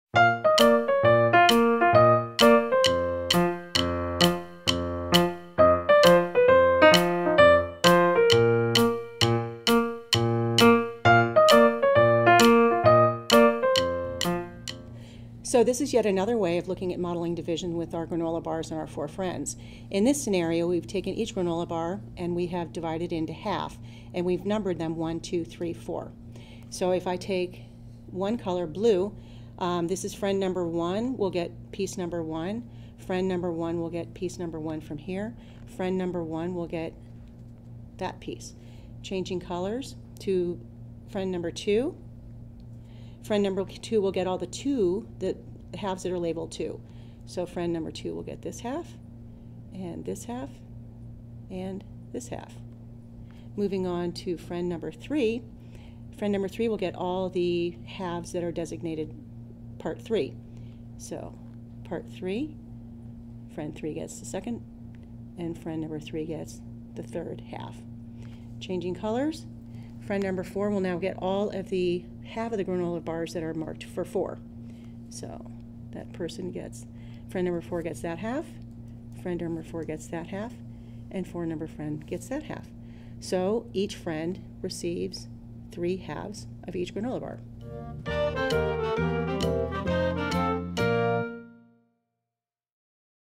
Everyday Mathematics: Modeling Division Example #3 - 3rd Grade
Ms. Blocks demonstrates Modeling Division.